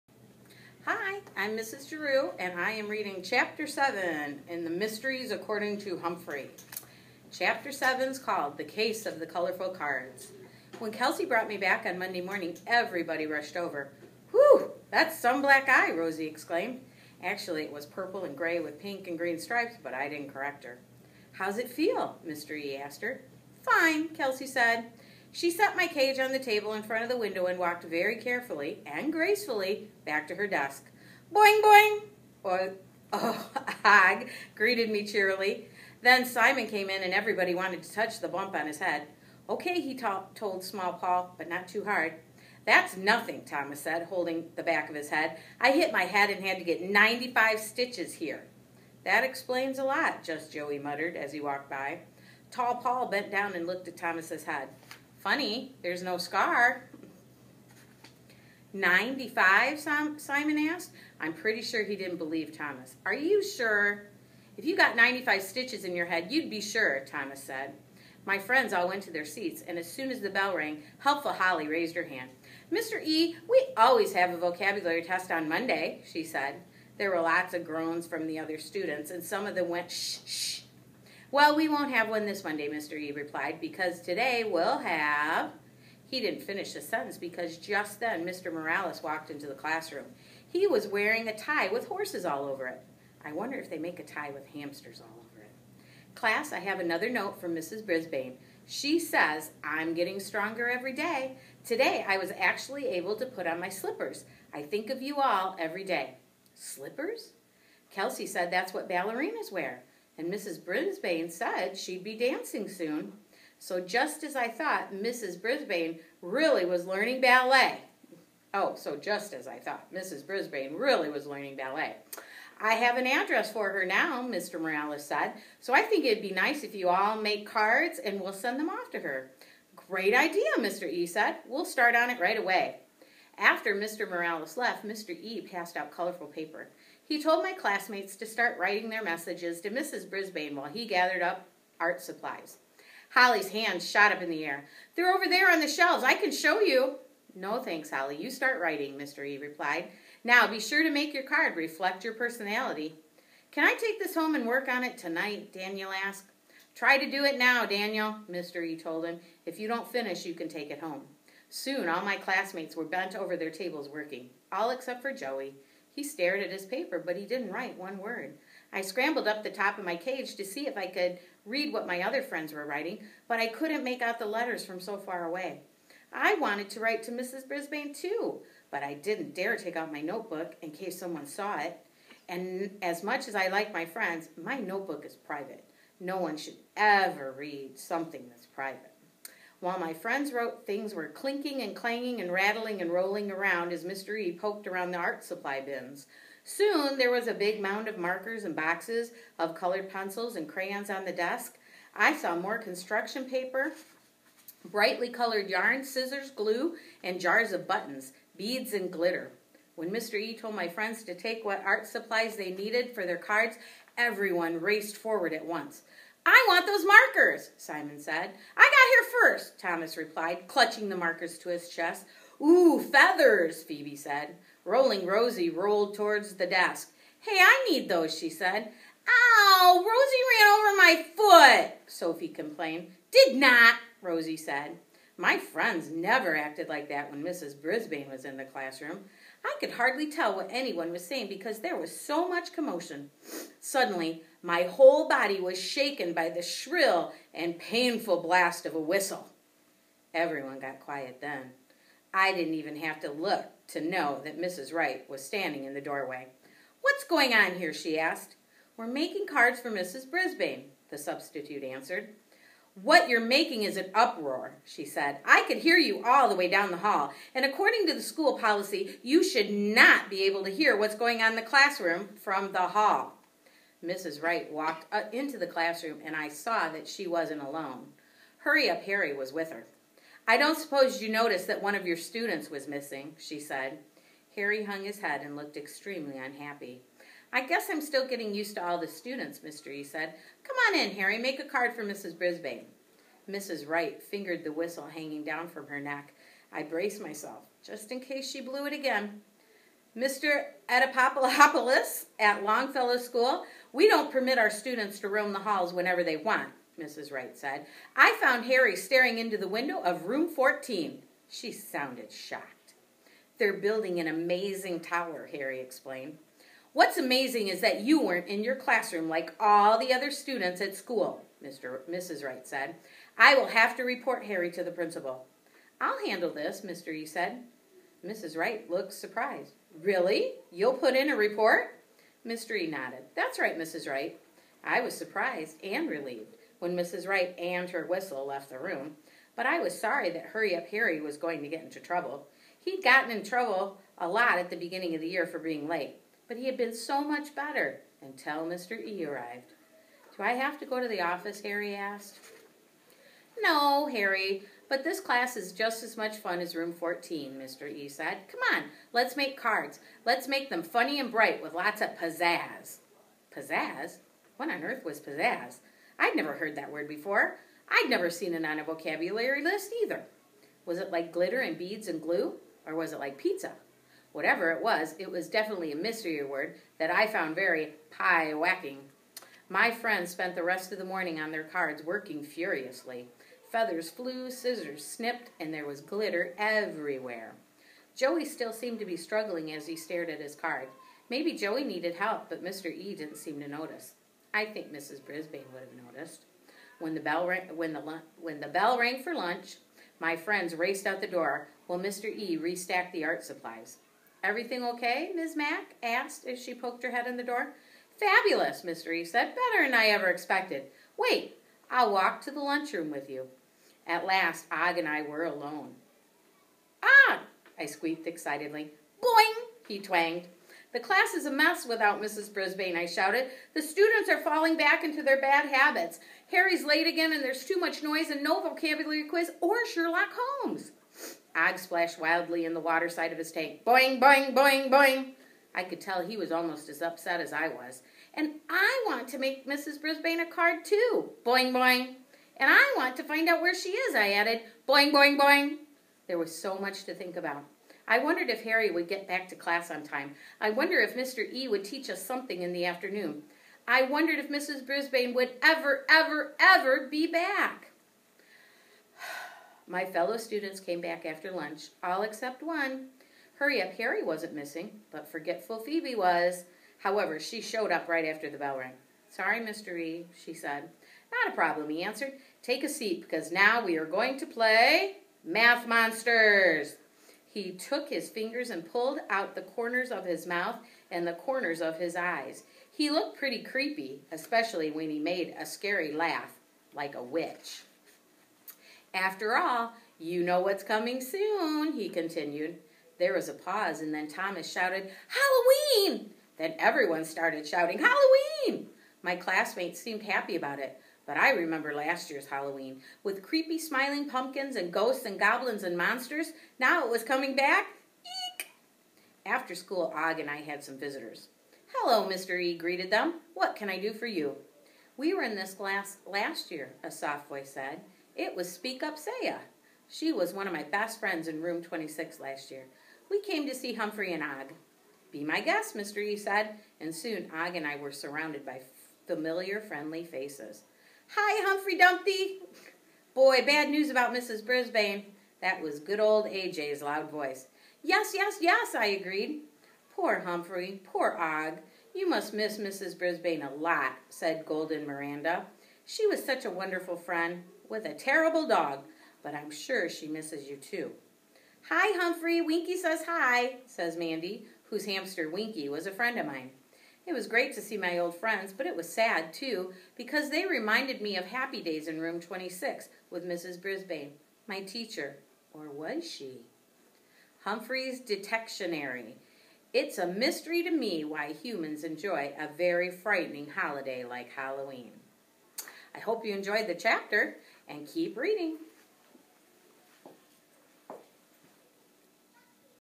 One Book, One School - Humphrey - Chapter 7
As part of the One School, One Book initiative as part of March is Reading Month, here is Mrs. Jerue reading chapter 7 of our book, Mysteries According to Humphrey. Enjoy!